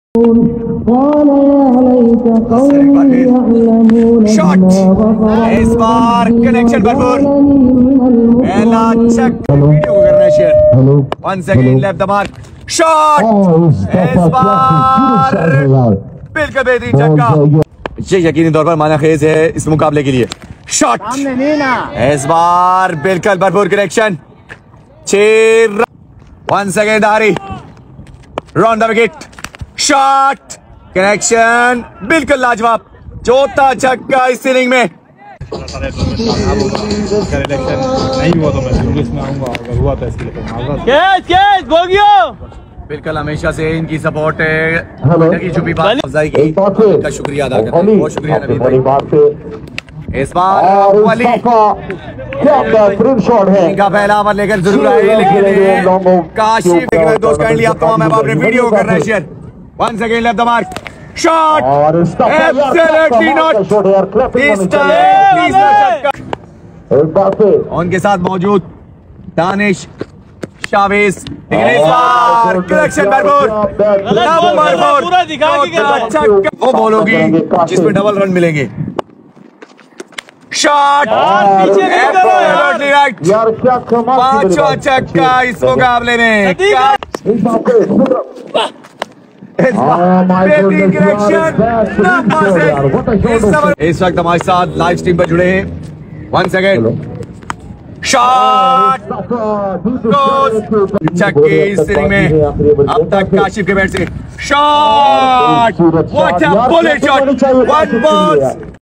Kashif Jokhio Batting in BMTCT ❤🔥🏏 | Tape Ball | Cricket
Kashif Jokhio Batting in Balaaj Memorial Tape Ball Cricket Tournament  ❤🔥🏏  | Tape Ball | Cricket